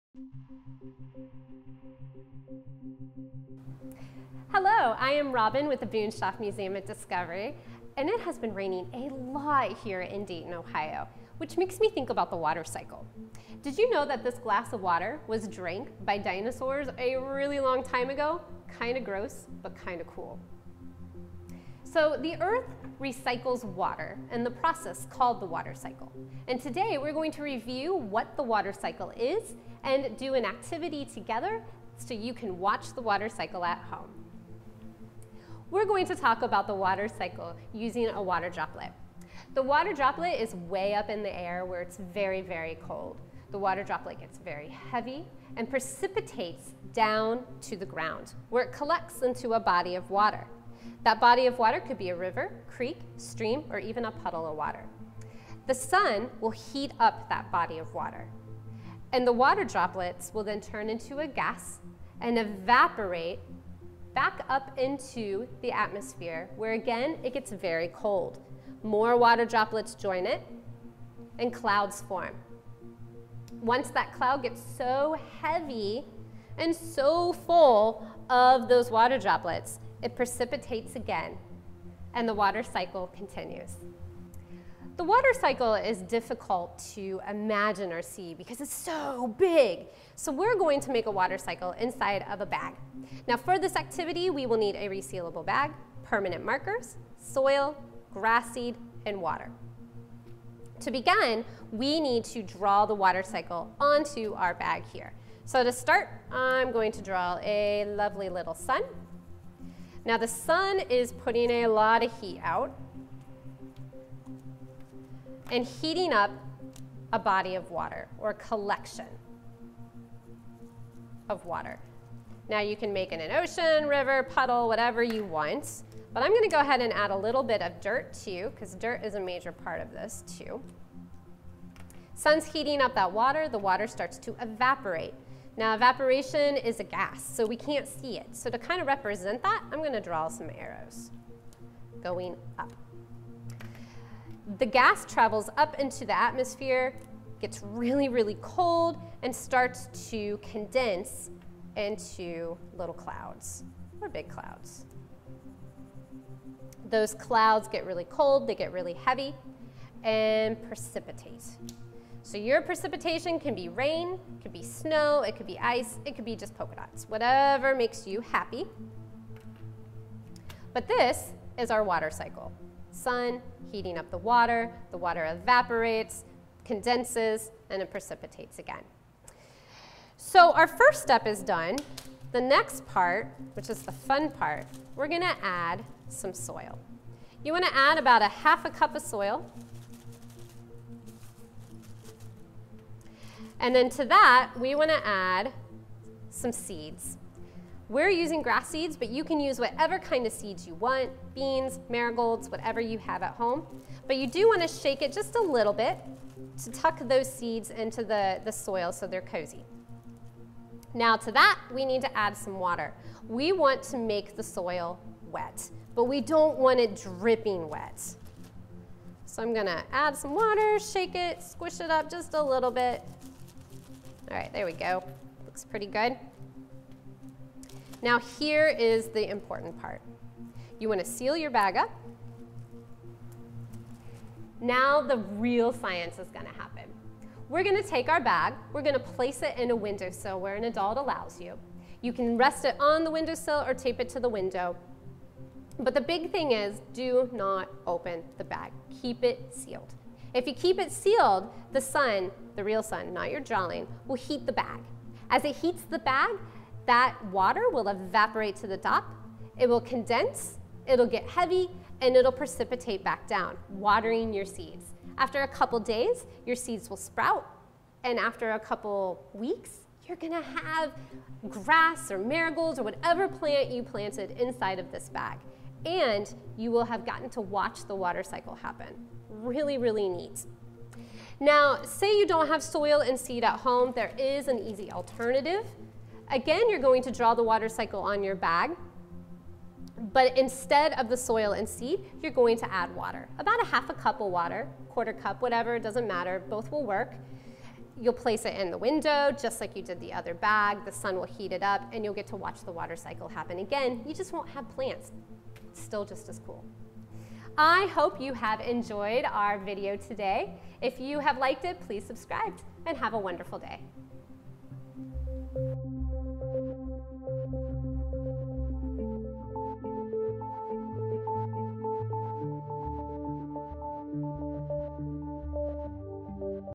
Boonshoft at Home - The Water Cycle
Robin talks about how the Earth recycles water with a fun experiment you can do at home!